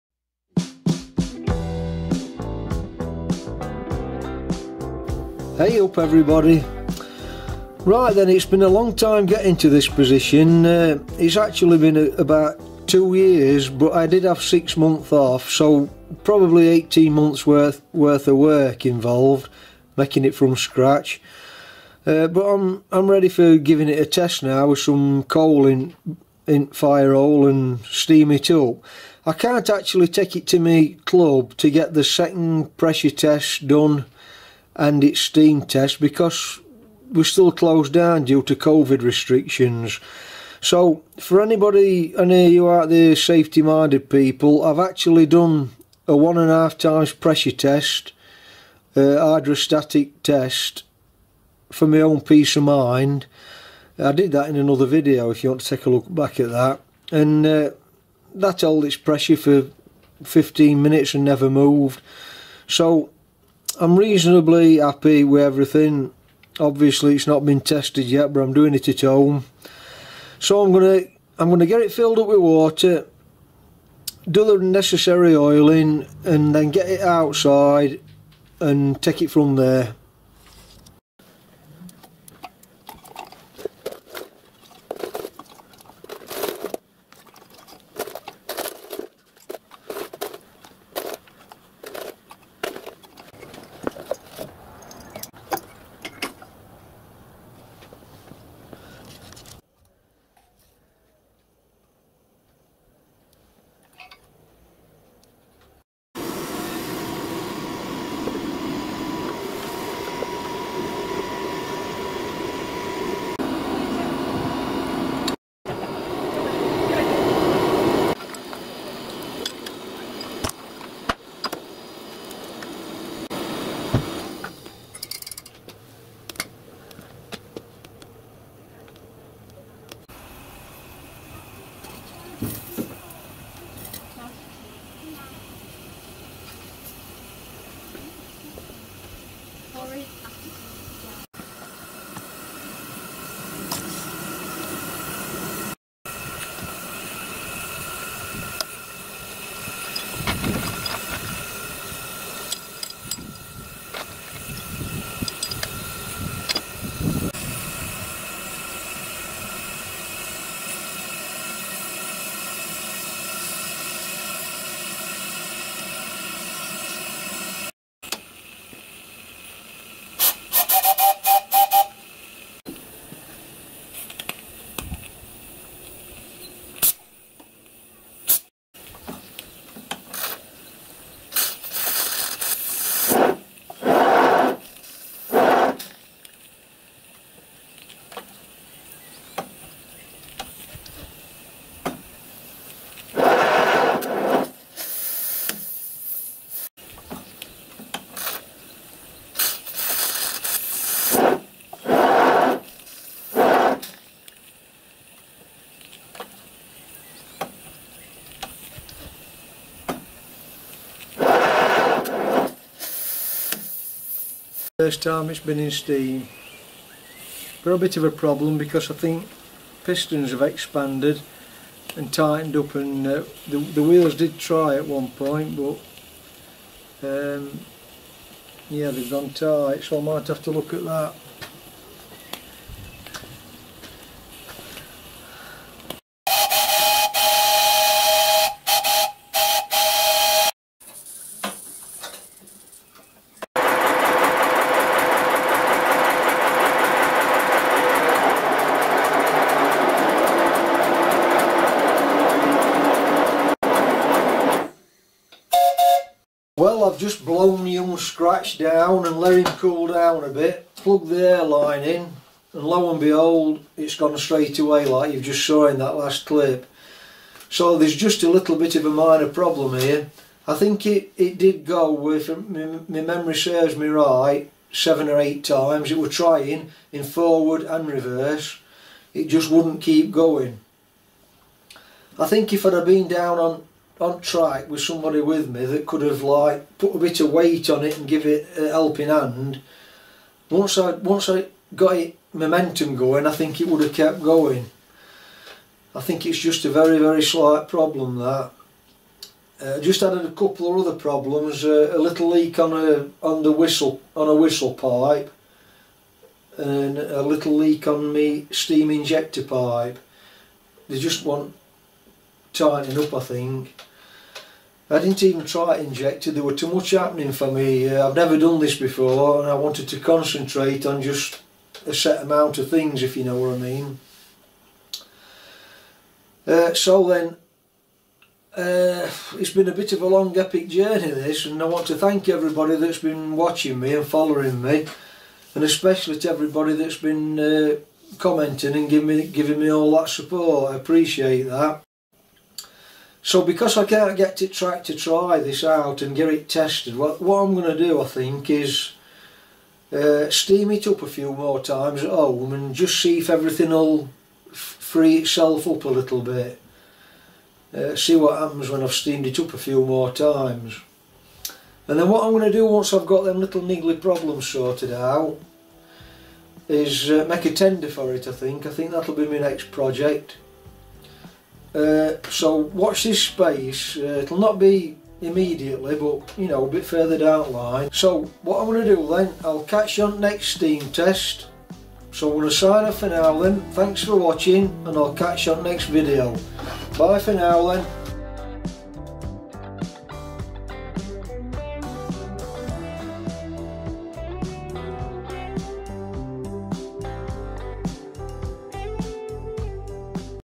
Build Your Own Beginners Steam Locomotive Series (1st Steam Test & Thankyou ) "mr factotum"
Scratch - 1st Fire Up under Steam & A Big Thankyou to all for watching and commenting
Continuation of my step by step Beginners Miniature Live Steam Locomotive build from scratch. A
0-6-2 Metre Maid version of a 0-4-0 Sweet Pea Locomotive
Here showing, First Home Steam Test to find any niggling faults
(( 2nd STEAM TEST COMING UP see how i overcome the niggling problem of the pistons nipping up under steam, quick and easy solution ))